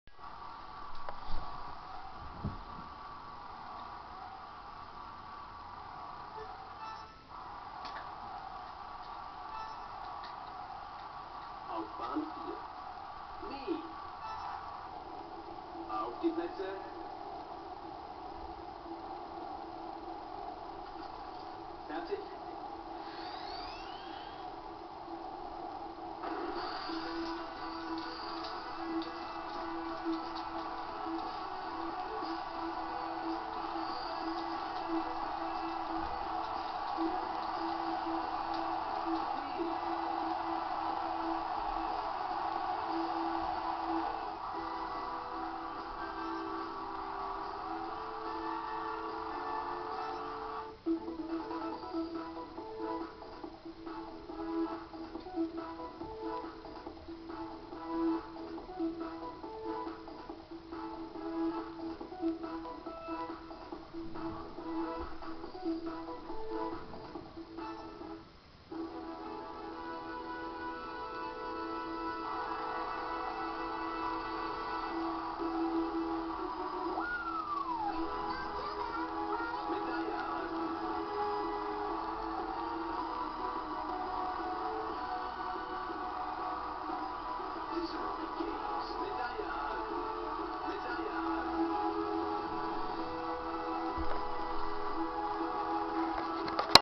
Olympische Spiele - 110m Hürden
ich in Beiing bei den olympischen Spielen 08 :-D